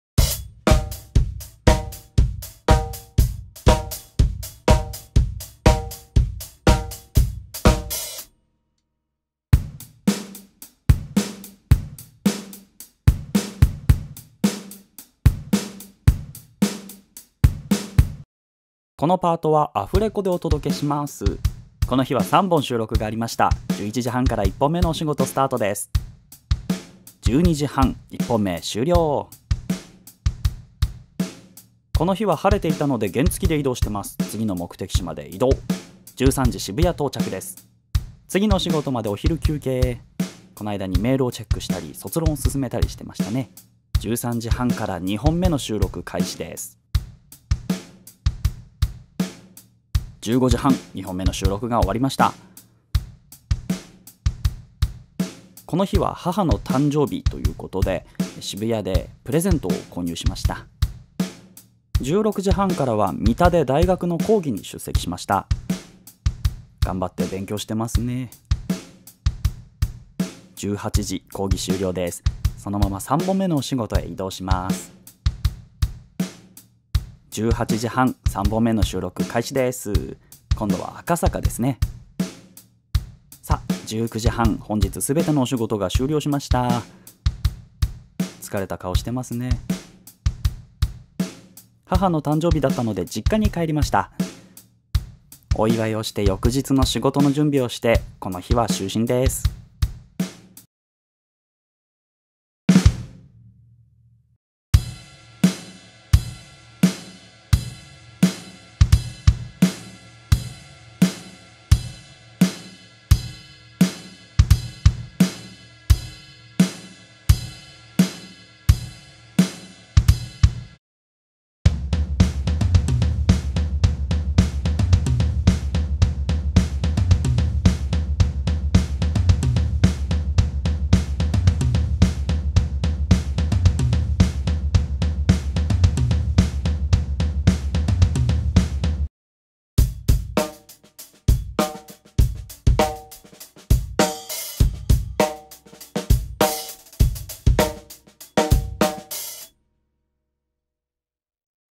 【声優大学生の日常】November 2022｜Vlog
田中光（タナカヒカル）です。
2018年から賢プロダクションというところで声優をしています。
あと、声優をしながら慶應義塾大学というところで大学生をしています。
”学生生活も残りわずか”ということで、ゆるーくvlogを始めてみました。
趣味で遊びで落書きです。期待せず肩の力を抜いて抜いて見ていってください。